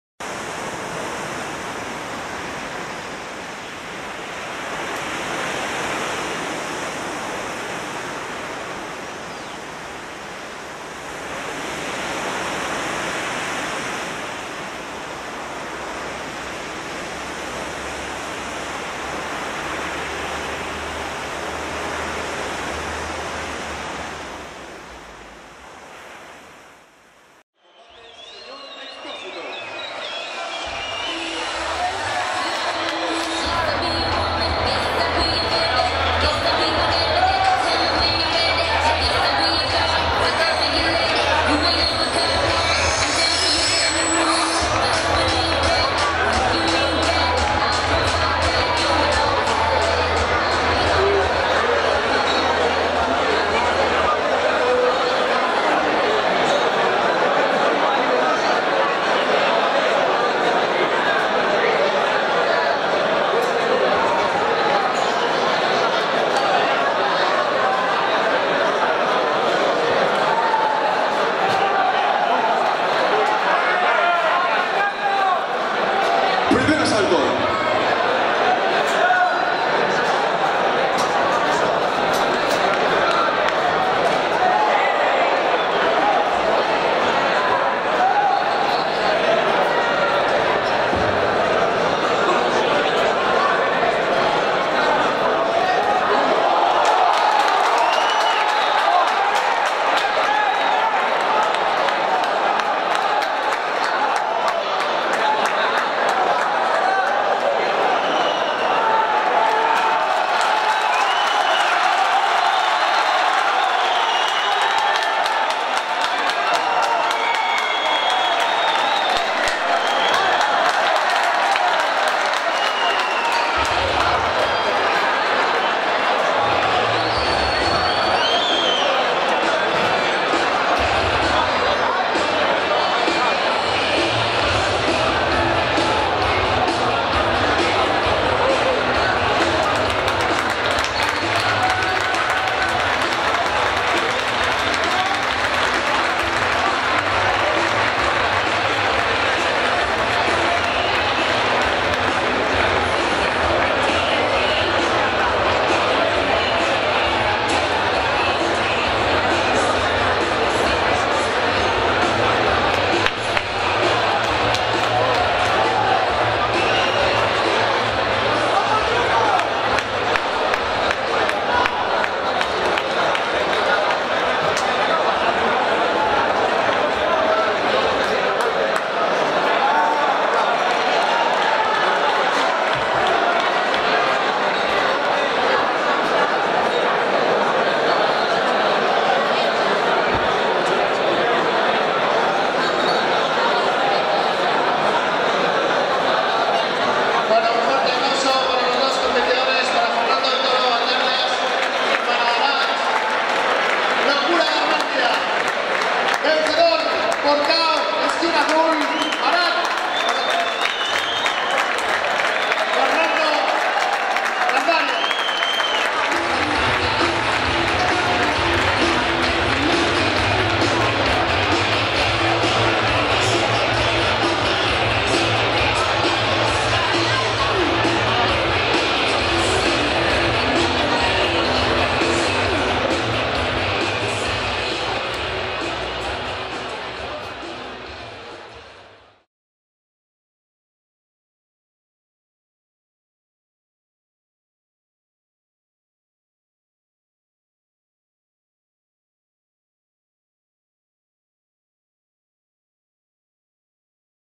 Fernando Gandarias vs Aratz Garmendia
Combate celebrado el 19/12/2015 en el Fronton Bizkaia de Miribilla ( Bilbao ) Organizado por MGZ Promotions